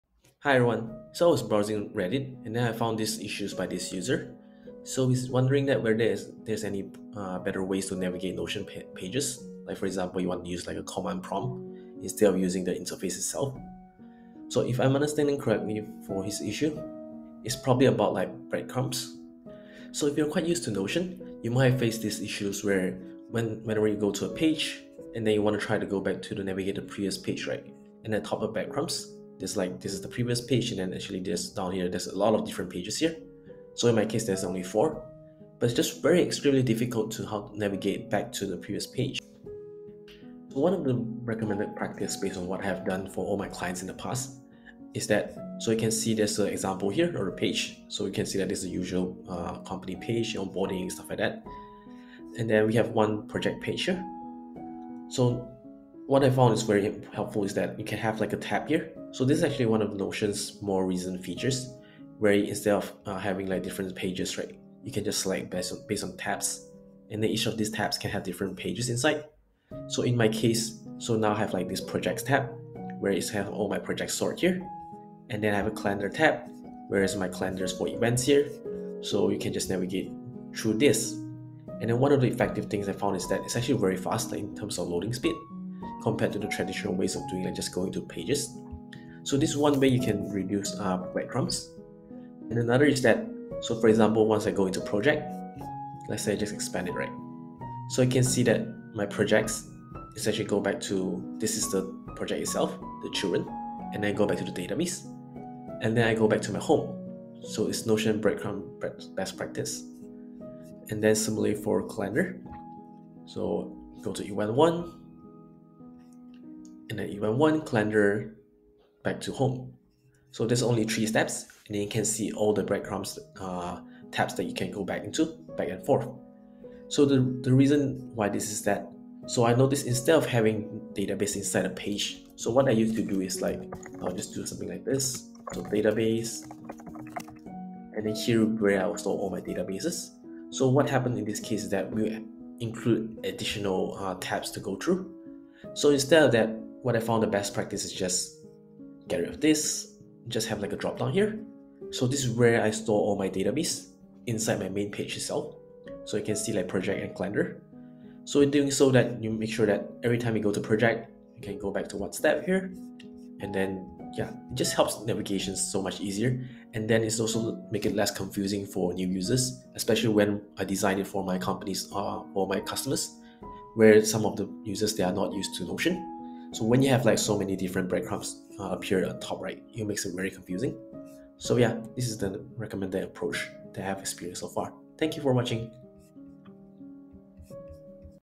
Q&A - Best Practice for Notion breadcrumbs
Hey all,

I saw this post on Reddit and decided to do a quick video to explain the best practice to structure your database and pages to allow easy navigation in Notion.